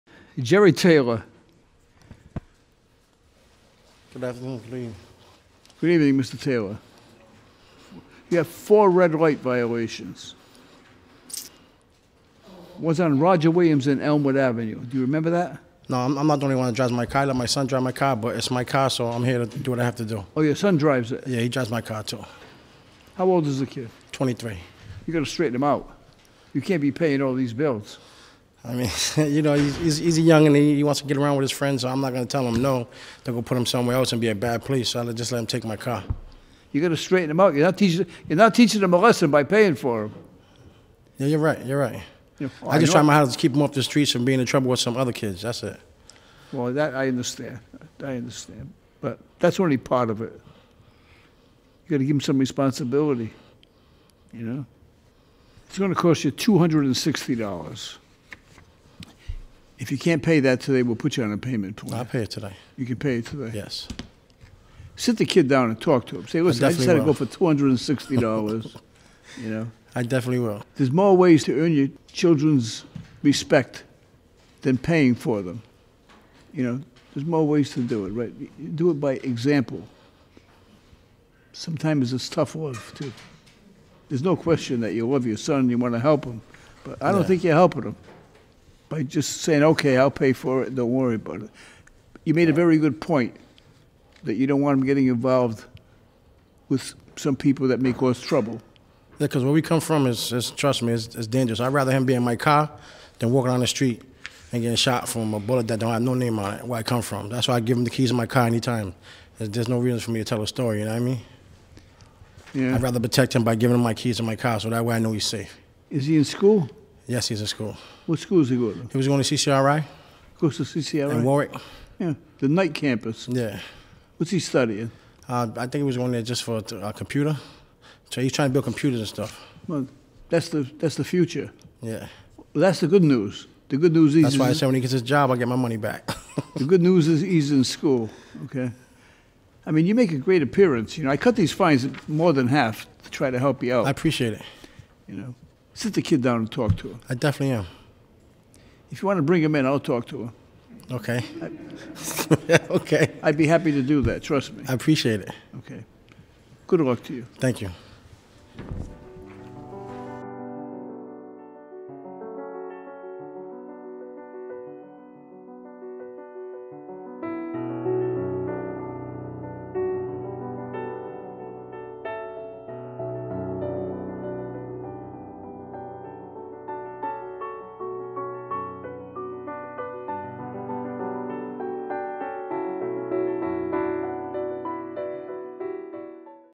Keeping Kids Off the Streets
Judge Caprio and a motorist discuss teaching children accountability and how to keep them safe from dangerous situations.